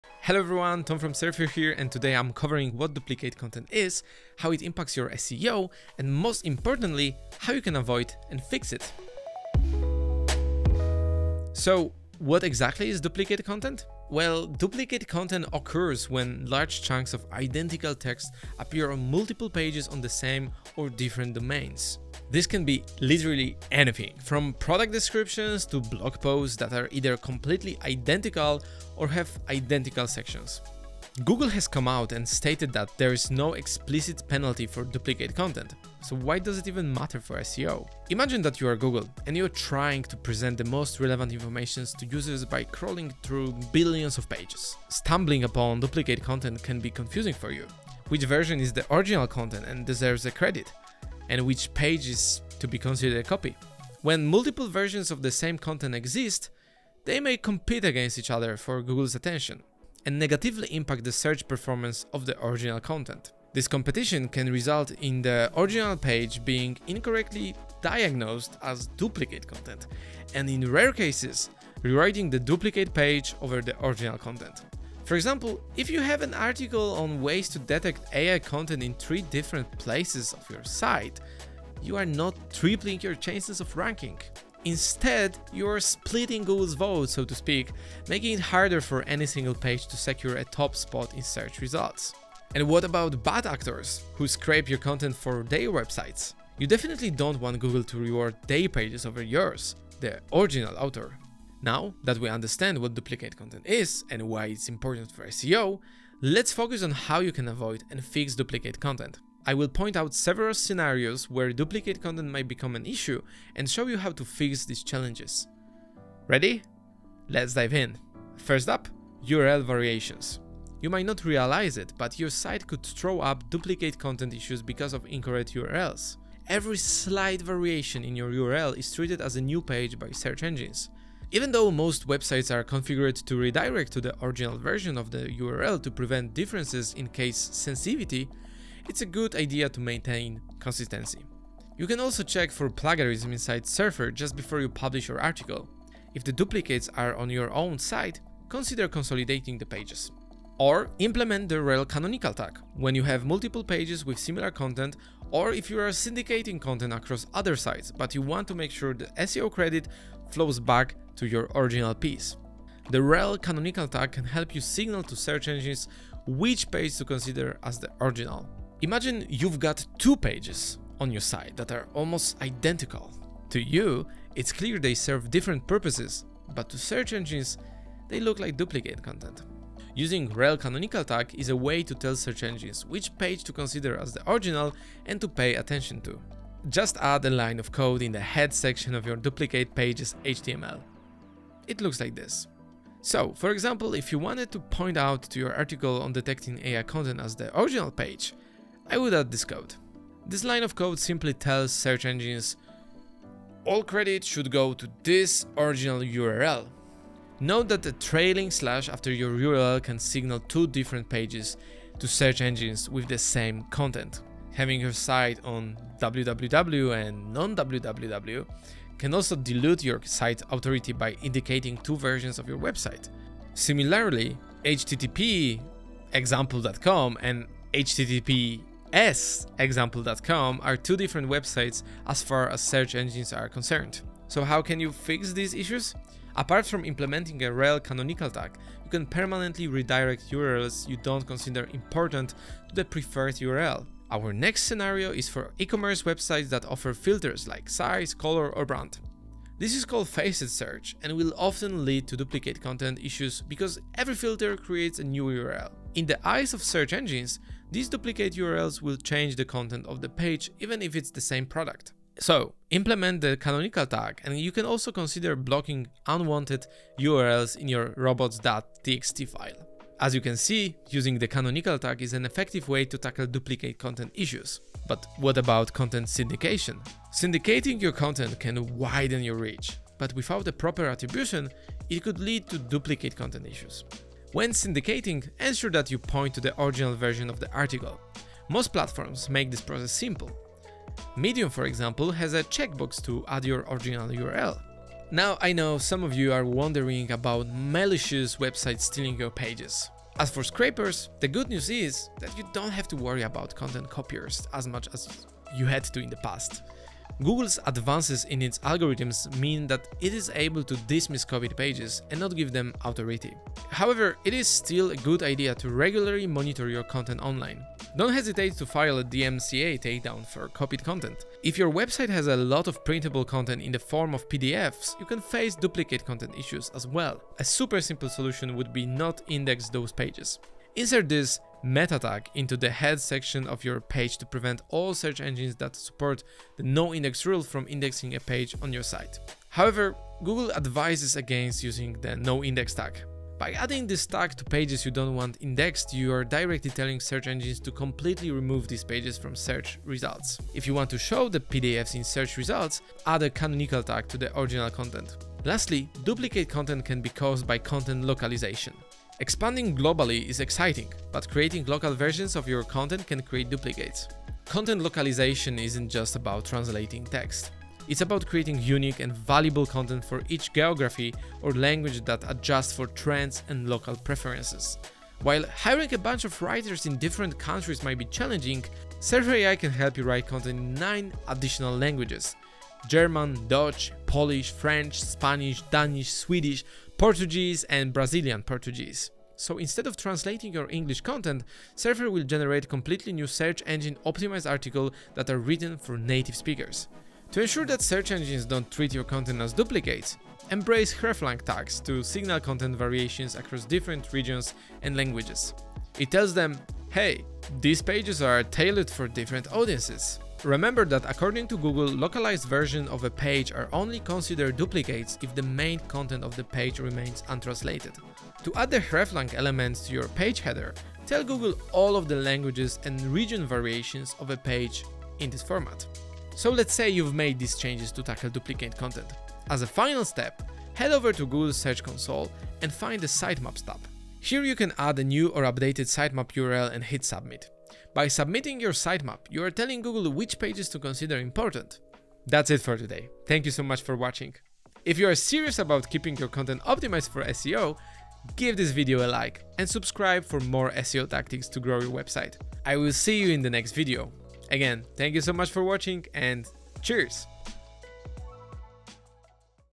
How You Can FIX and AVOID Duplicate Content Like a PRO!
🚀 Tired of duplicate content hurting your SEO? Learn how to identify, avoid, and fix it with tips from Tomasz Niezgoda, Surfer's Head of Marketing! 🛠️

Implement canonical tags, consistent URLs, and more. Keep your content optimized and boost your rankings! 🌟

What's on the agenda:
00:00 - 01:57 What is Duplicate Content? Why You Should Care?
01:58 - 02:29 URL Variations
02:30 - 04:03 Canonical Tags
04:04 - 04:34 E-commerce and Faceted Search
04:35 - 04:55 Content Syndication
04:56 - 05:21 Dealing with Scrapers
05:22 - 05:55 PDFs and Noindex Tags
05:56 - 07:05 Content Localization
07:06 - 07:40 Final Steps